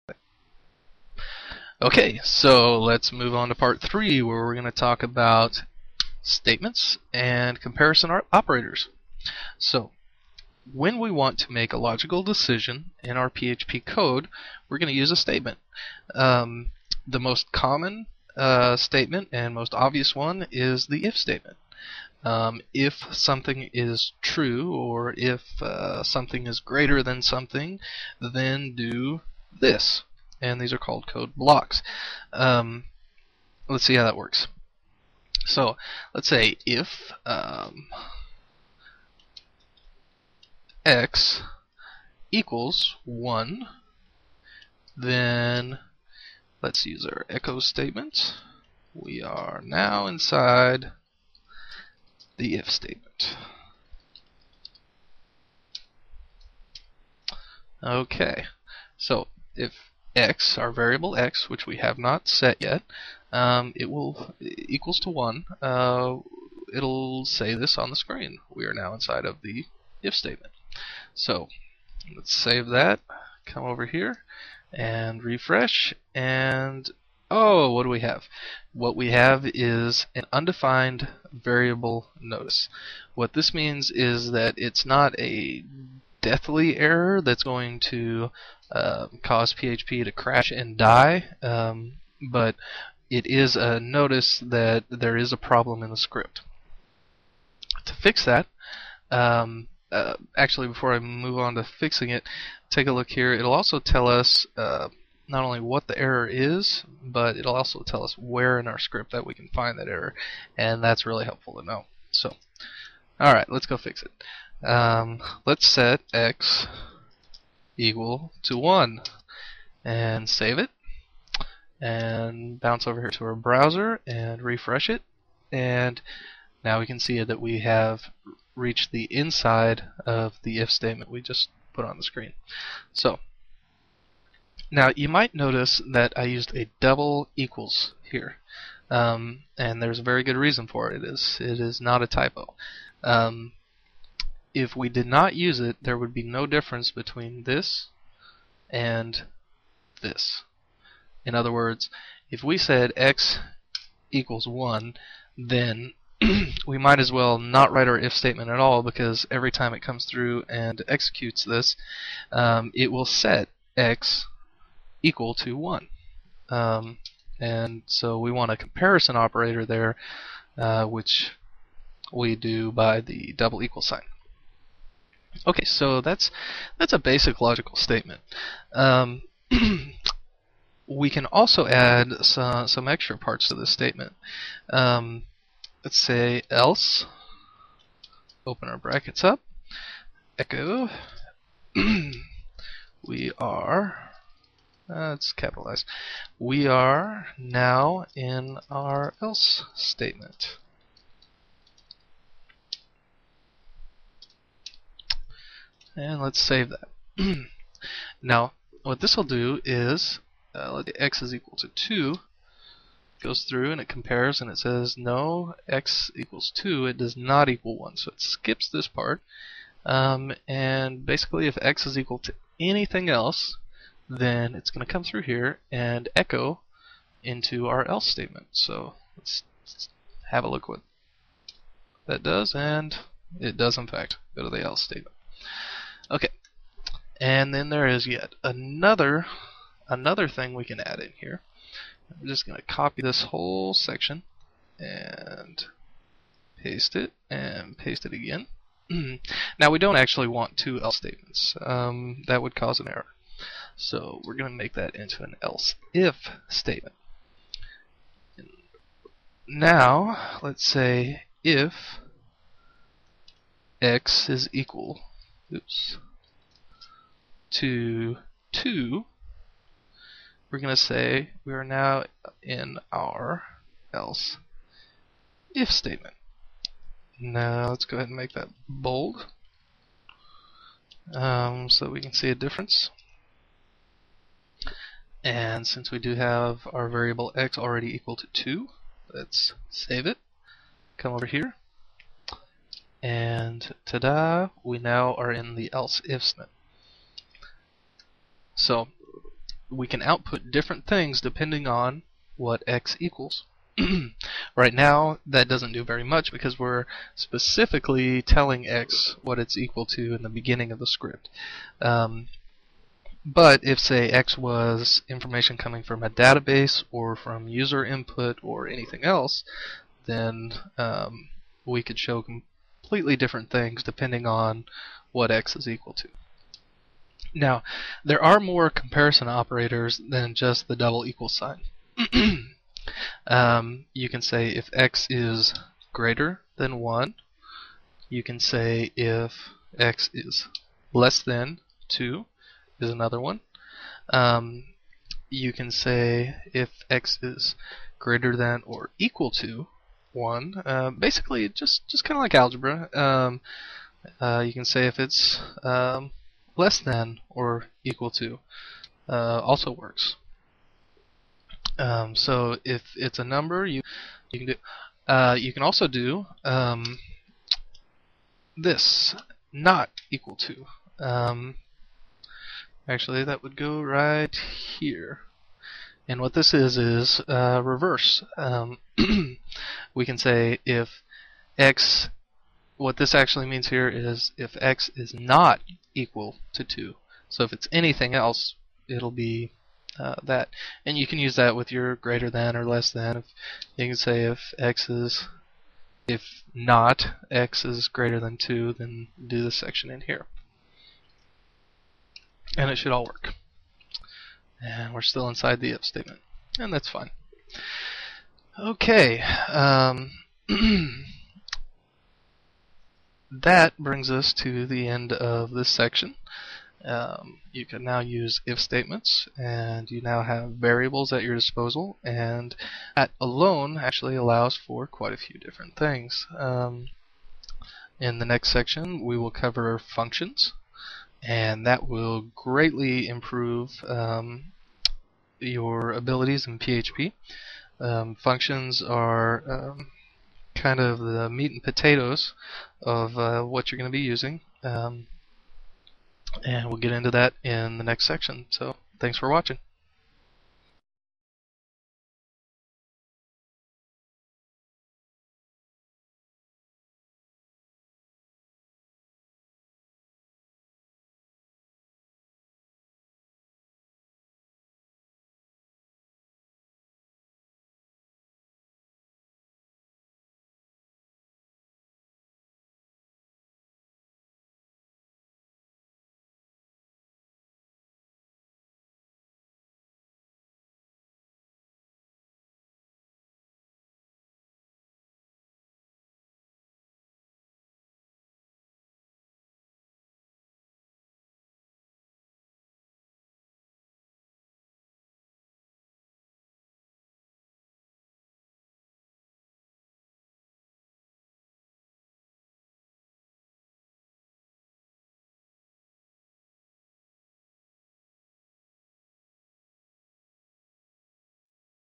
Chapter 3 PHP Editors, Comments, and Basic Variables
Another step down the path of learning PHP programming.

In this chapter we get into the nuts and bolts of PHP.. commenting, variables, and choosing a code editor.